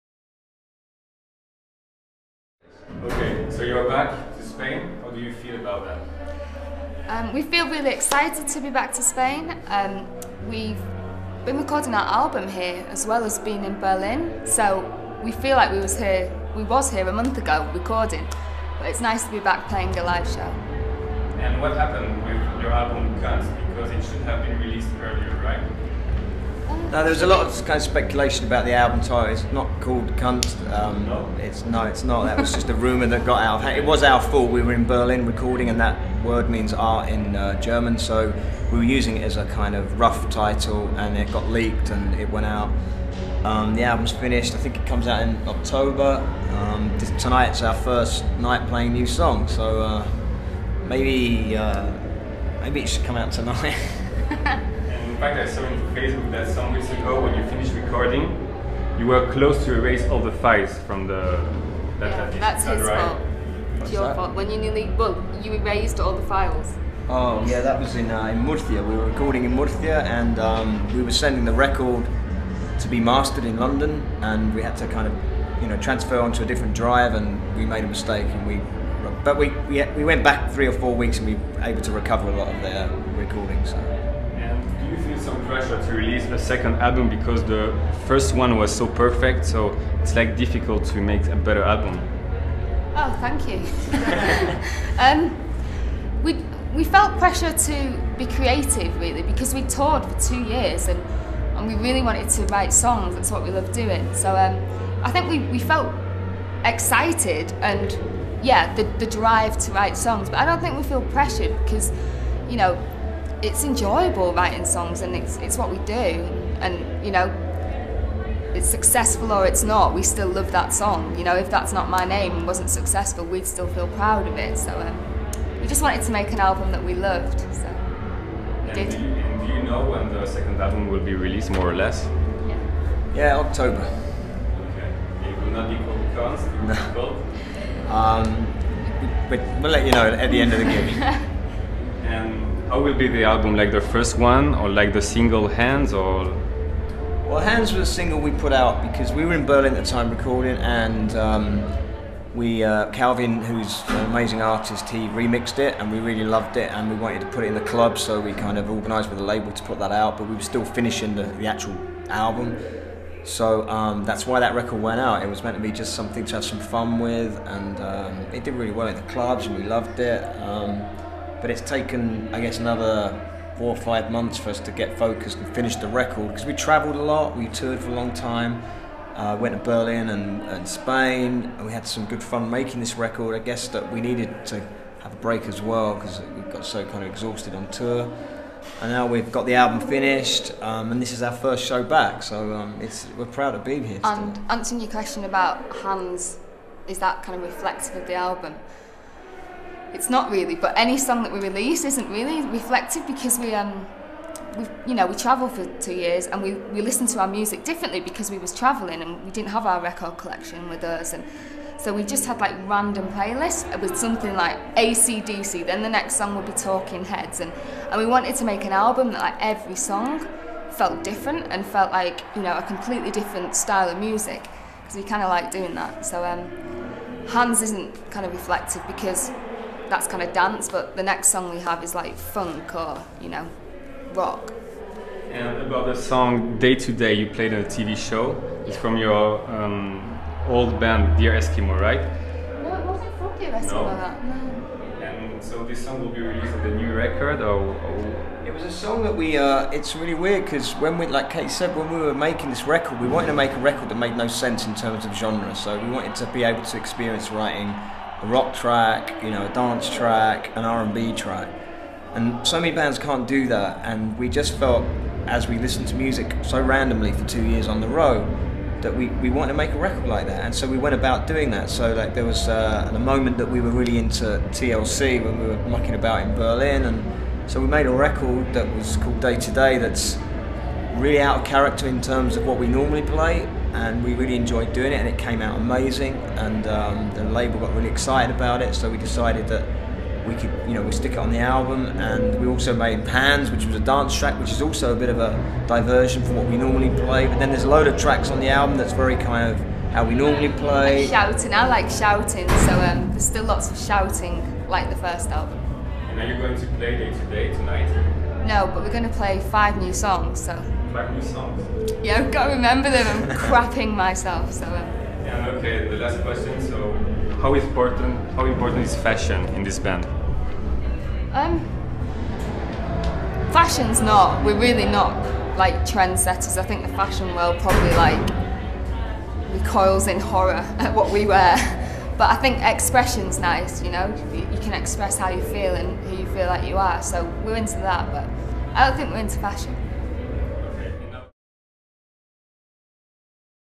Entrevista a Ting Tings - dcode Festival 2011
Durante su paso por los escenarios del dcode, conseguimos robarle unos minutos al dúo mas fashion de inglaterra para que nos contasen novedades sobre el futuro más inmediato de la banda.